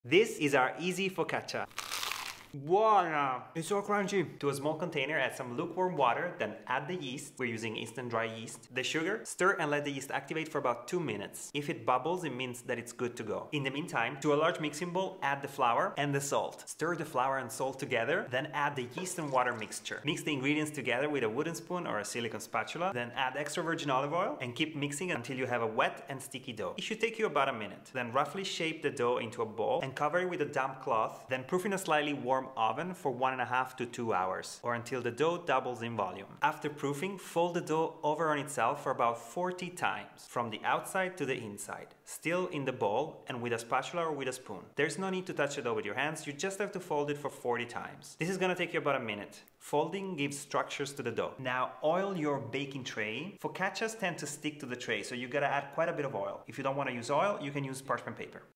Easy Focaccia Bread pt. 1
⭐️  Focaccia bread is so easy! All you need is flour, water, salt, and yeast for the dough, and rosemary and olive oil as toppings. You can make it on the day or get ahead and let it proof slowly in the fridge.

⭐️ Ingredients
3½ cups flour all-purpose flour or half all-purpose and half whole wheat flour
1½ cups + 1 tablespoon lukewarm water
1 teaspoon yeast instant dry yeast or active dry yeast. 10 grams if you use fresh yeast.
1 teaspoon sugar
2 teaspoons salt

Theplantbasedschool.com